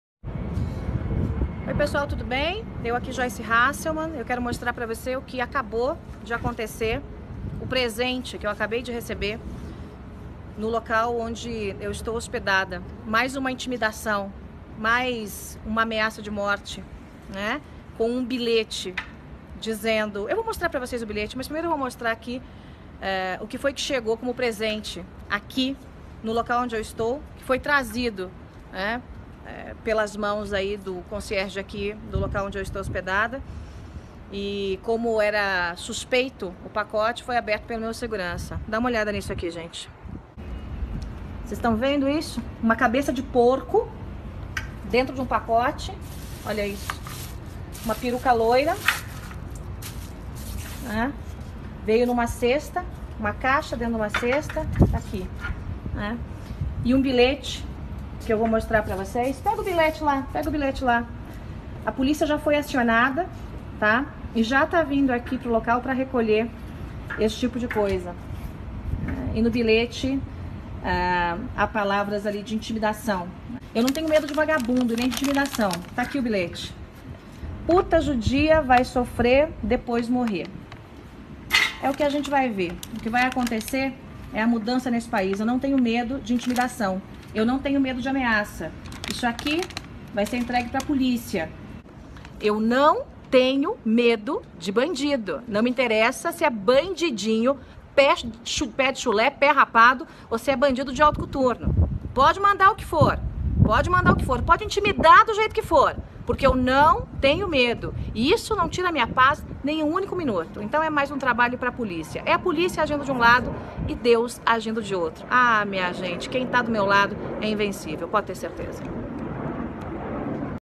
Joice Hasselmann recebe cabeça de porco com bilhete de ameaça de morte
Joice Hasselmann foi ameaçada de morte nesta sexta-feira (30). A encomenda trazia uma cabeça de porco, uma peruca loira e uma carta escrita com recortes de revistas com a mensagem “P* judia, vai sofrer, depois morrer”.

– Não tenho medo de vagabundo nem de intimidação. que vai acontecer é a mudança nesse país. Isso não tira minha paz em nenhum único minuto. É mais um trabalho para a polícia. A polícia agindo de um lado e Deus agindo de outro. Quem está do meu lado é invencível – declarou Joice.